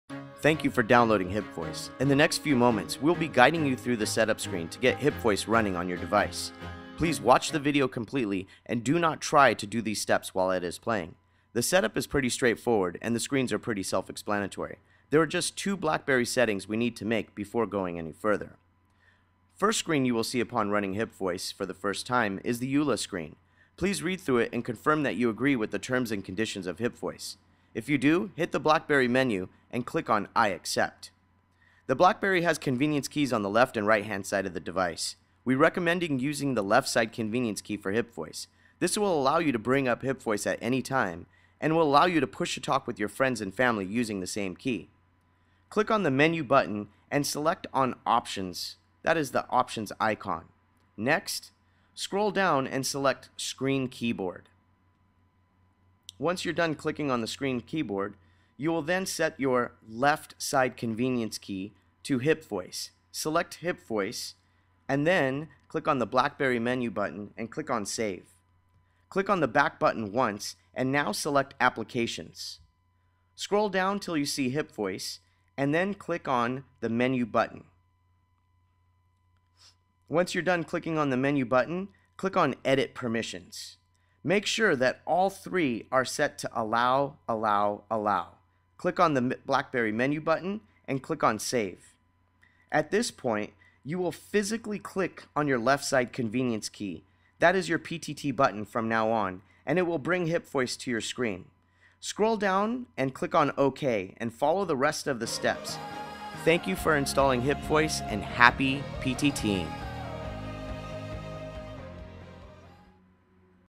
HipVoice BlackBerry 5 Setup Video
Learn how to setup HipVoice on your BlackBerry 5.0+ device.